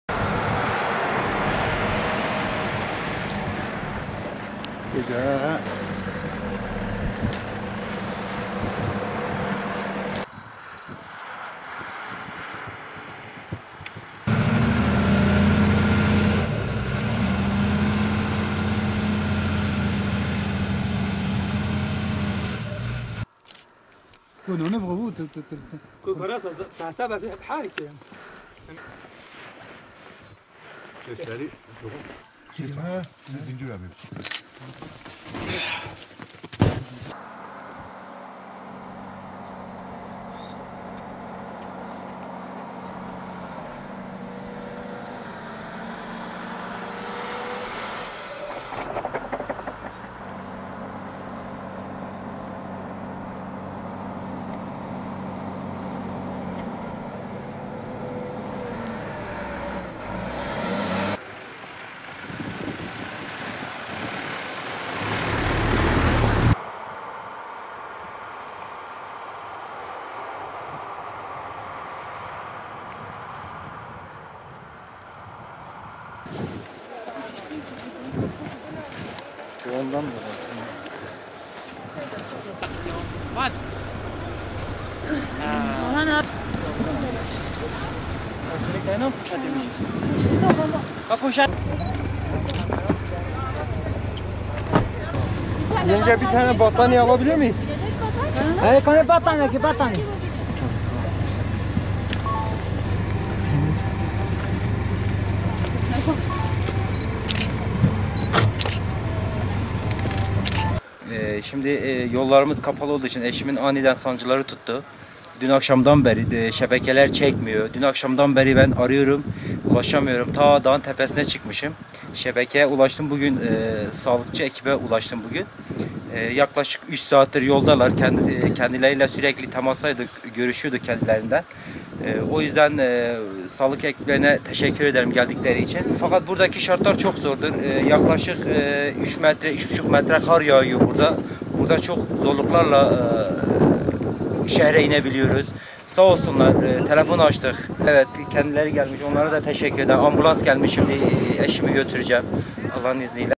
Ekipler, hamile kadın için seferber oldu
HAKKARİ’nin Yüksekova İlçesi'ne bağlı yolu kar nedeniyle kapalı Yukarı Uluyol Köyü'nde doğum sancıları başlayan Elvan Kayacı Uçun'a ulaşabilmek için ekipler seferber oldu. 3 saat süren yol açma çalışmaları sonucu köye ulaşan ekipler, Uçun'u alarak ambulansla hastaneye ulaştırıldı.